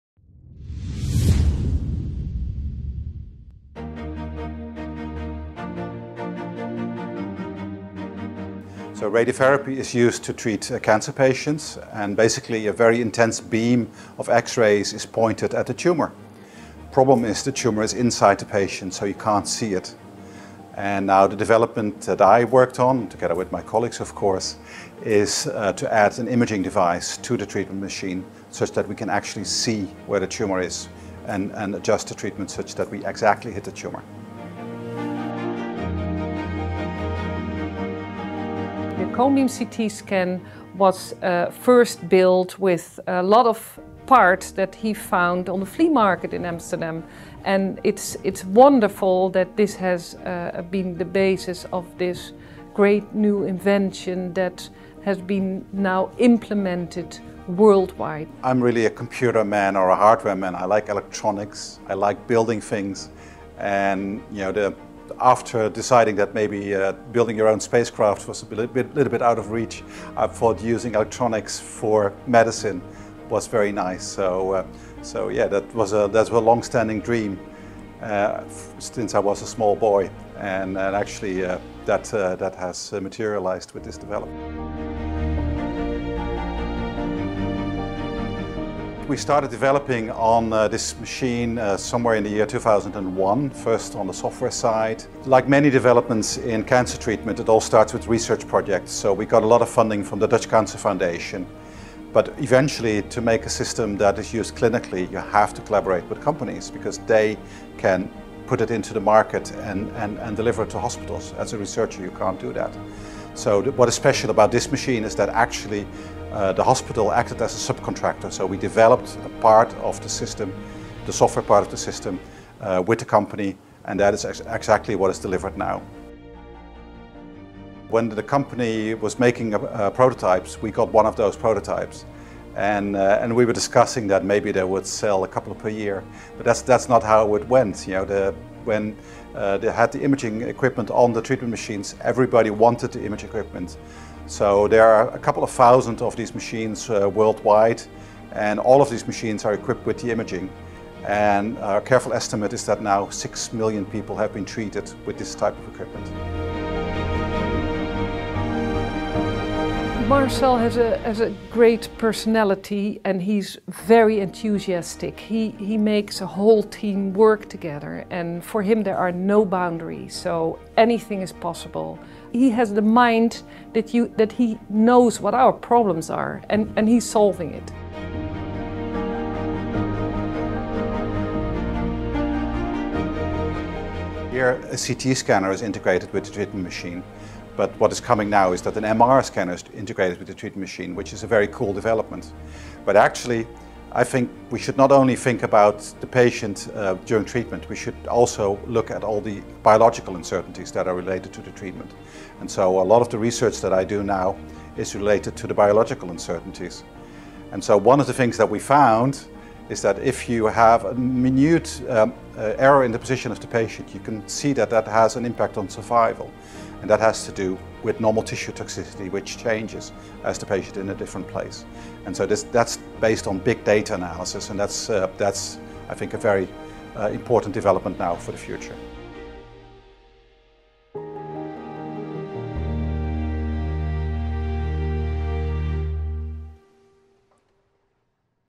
Impact Award Marcel van Herk - NCI/AvL
The Impact award is an award for the ‘established’ researchers, who have already acquired a track record in the field of valorisation. With this Impact Award, we would like to honor the researchers who have made a meaningful contribution to society, with their important innovations.

The nominees are:

Beta/life sciences
René Bernards | Netherlands Cancer Institute/Antoni van Leeuwenhoek | Molecular Carcinogenesis
Marcel van Herk | Netherlands Cancer Institute/Antoni van Leeuwenhoek | Radiotherapy
Sophia Kramer | Amsterdam UMC – locatie VUmc | KNO-Hoofdhalschirurgie, sectie Ear & Hearing – Amsterdam Public Health research institute
Philip Scheltens | Amsterdam UMC – locatie VUmc | Neurologie – Alzheimercentrum

Alpha/gamma
Erik Scherder | Vrije Universiteit Amsterdam | Faculteit Gedrags- en Bewegingswetenschappen IBBA: Institute for Brain and Behavior Amsterdam
Peter Weijs | Hogeschool van Amsterdam |Bewegen, Sport en Voeding